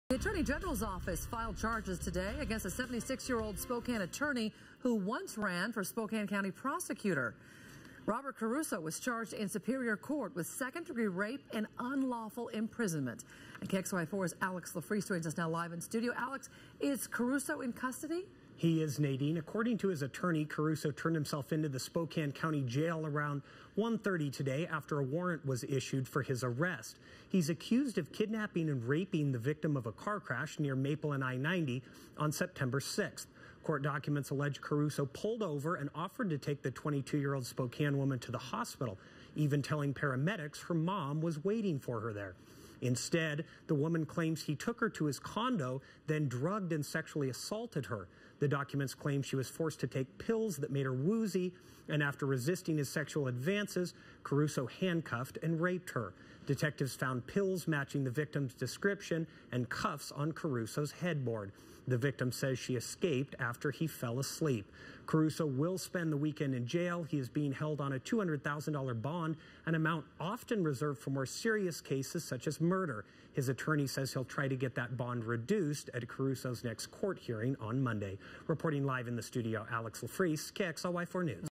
Spokane attorney charged with rape
Robert Caruso, who once ran for Spokane County Prosecutor, is now he's  facing prosecution over a rape charge involving a 22-year-old woman. KXLY4's Alex LeFriec reports.